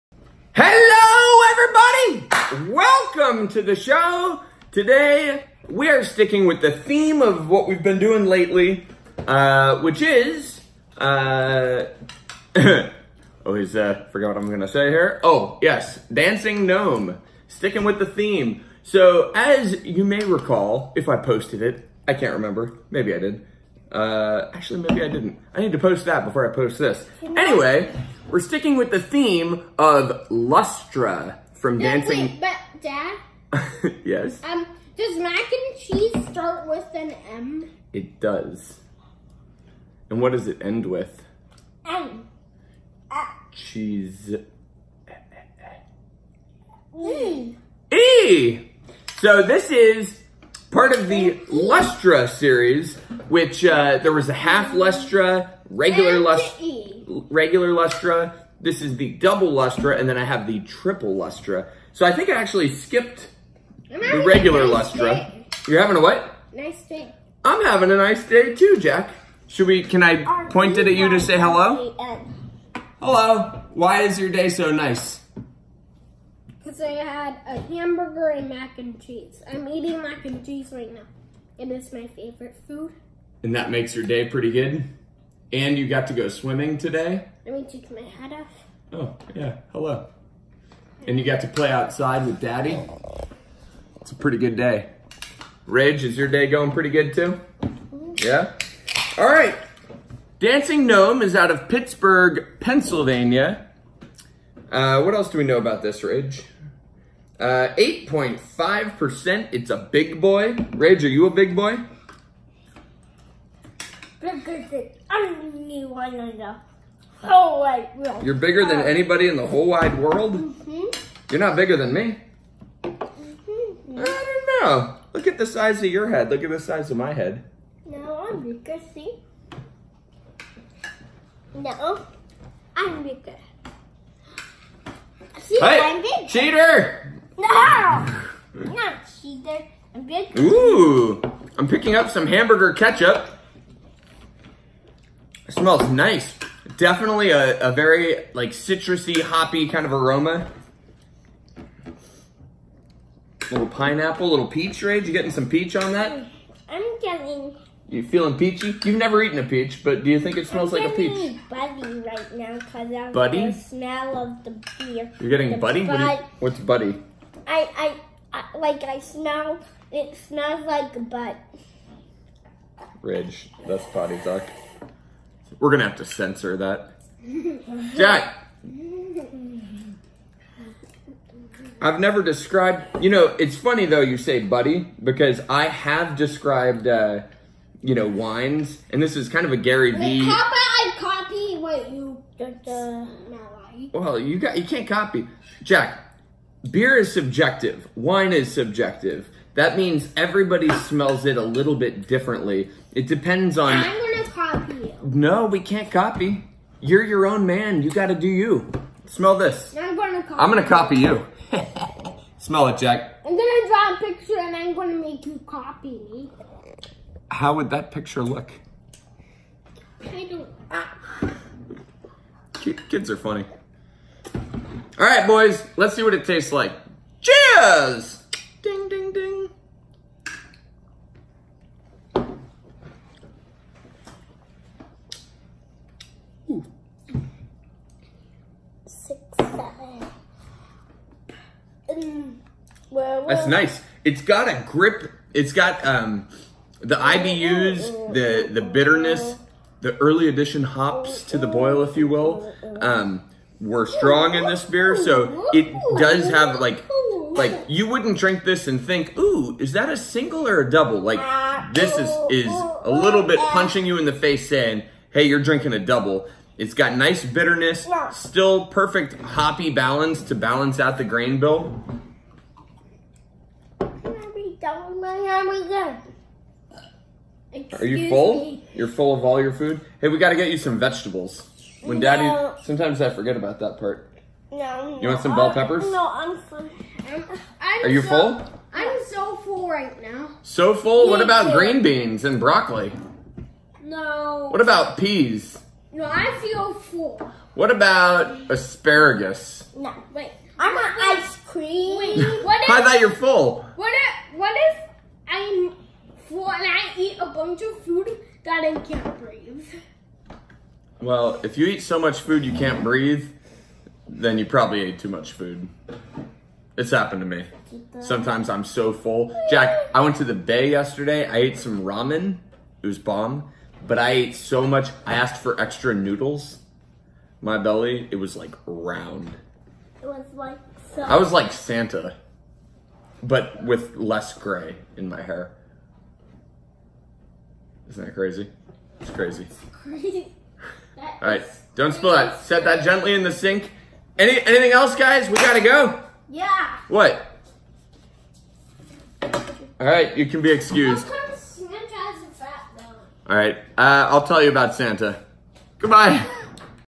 Double Lustra Beer Review from Dancing Gnome
This beer review video features another gem from Pittsburgh PA.  Dancing Gnome make really solid beers, but doesn’t get the same hype and attention as other breweries across the US.  This beer was big, but balanced.  Cheers!